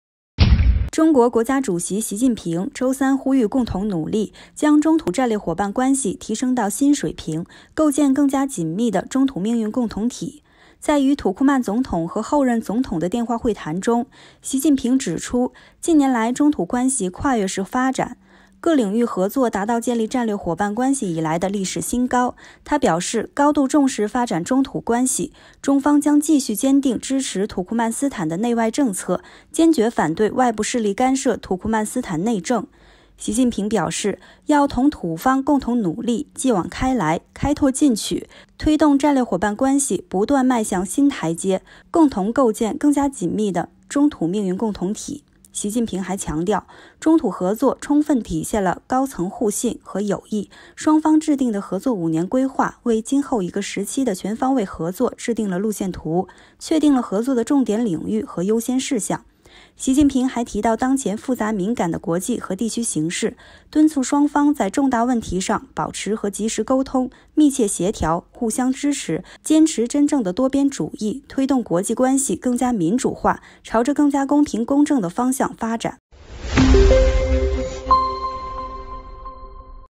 习近平同土库曼斯坦现任总统、候任总统通电话
中国国家主席习近平周三呼吁共同努力，将中土战略伙伴关系提升到新水平，构建更加紧密的中土命运共同体。在与土库曼总统和后任总统的电话会谈中，习近平指出，近年来，中土关系跨越式发展，各领域合作达到建立战略伙伴关系以来的历史新高。他表示高度重视发展中土关系，中方将继续坚定支持土库曼斯坦的内外政策，坚决反对外部势力干涉土库曼斯坦内政。习近平表示，要同土方共同努力，继往开来，开拓进取，推动战略伙伴关系不断迈上新台阶，共同构建更加紧密的中土命运共同体。